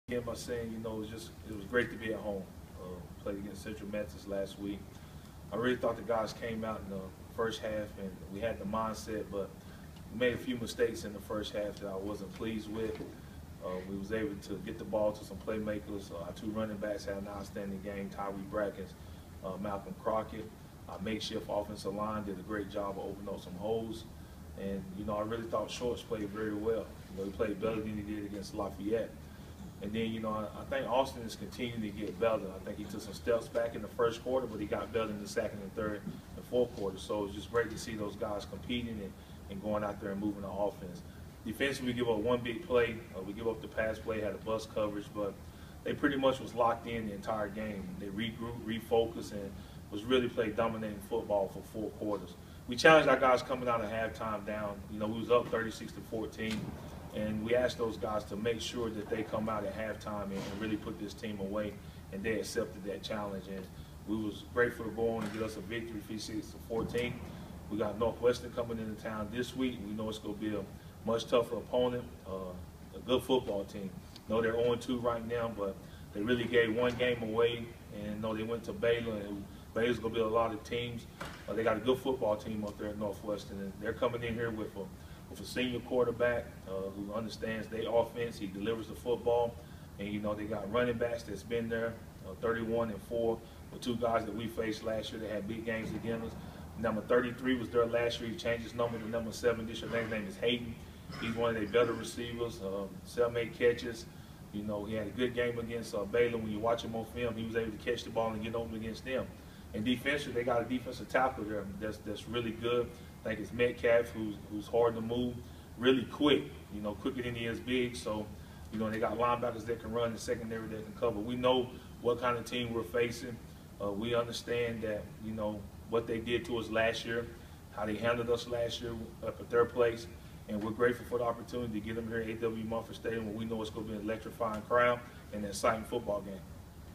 Southern University coach Dawson Odums liked what he saw in Saturday victory: Video
Southern University got just what the doctor ordered in its 56-14 victory against NAIA Central Methodist on Saturday. The Jaguars made some mistakes, Southern coach Dawson Odums, but he saw a lot of good things on which to build. He talks about that